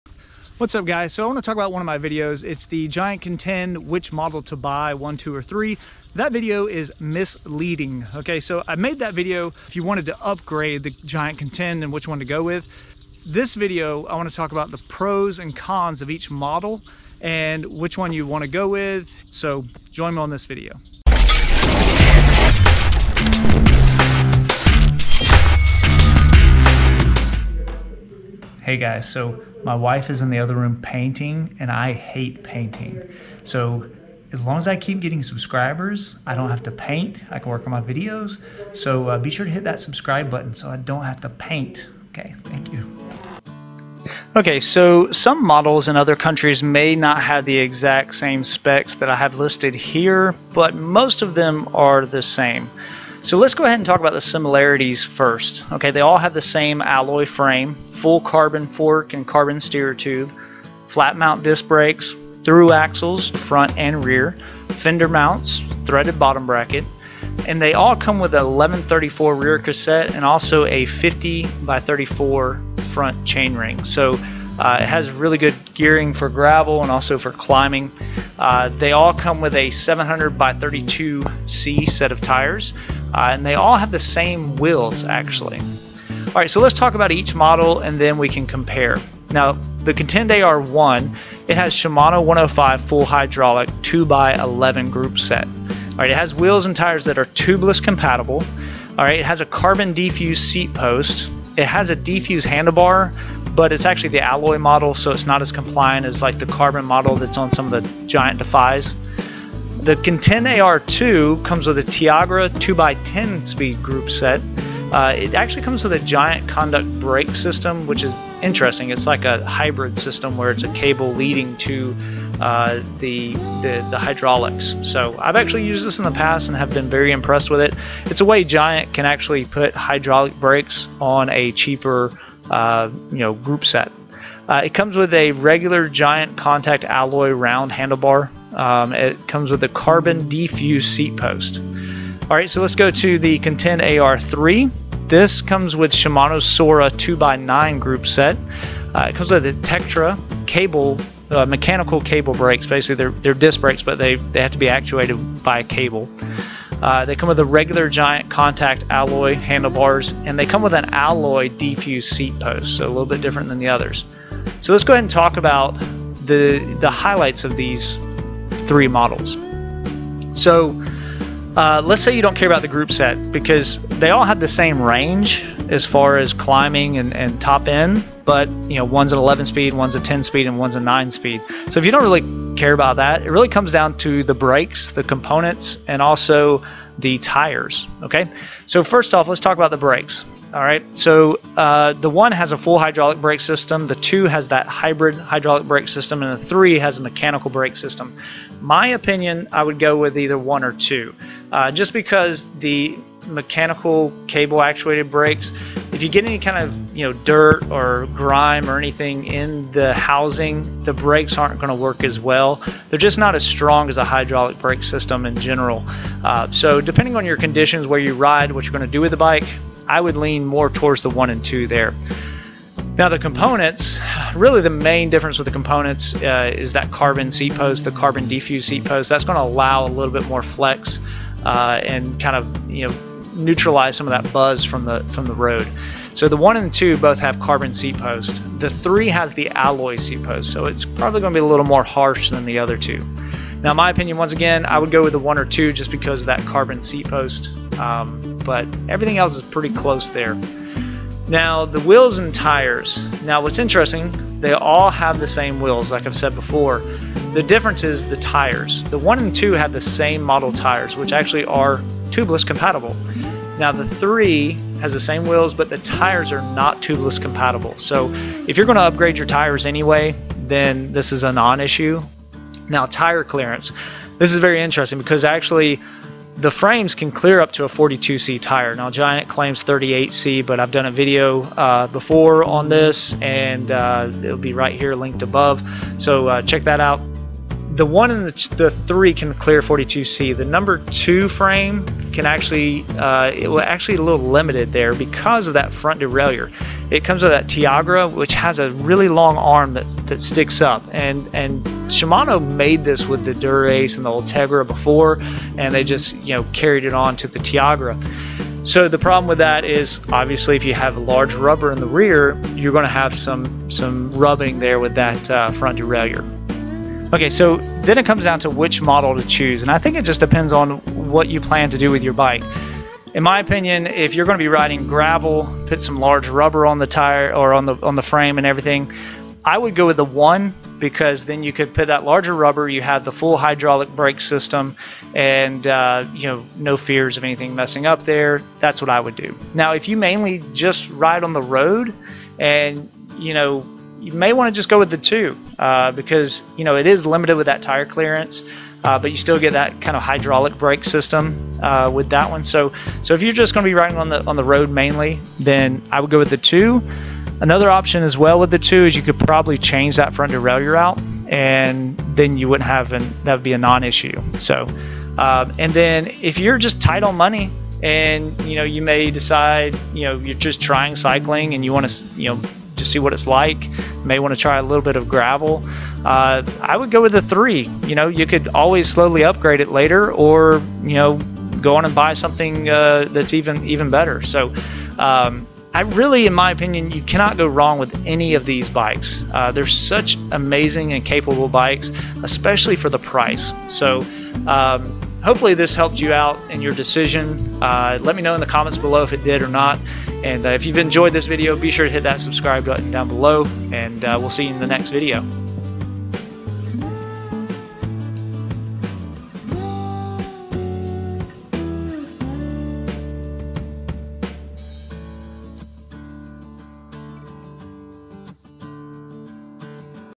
NEW IMPROVED | 2020 Giant Contend AR | Which Model to Buy 1, 2, or 3?
In this video I will do a quick overview of the 3 Giant Contend AR models and which one is best for what you plan to do with the bike. I re-did this video from one of my previous videos where I talked about which Contend to go with if you planned to upgrade the bike. This video is to inform you about the pros and cons of each model if you plan to keep the bike stock.

If you are new to my channel, I put up weekly videos on anything cycling related from tech/bike reviews, build tips, fitness, hacks, and race reviews.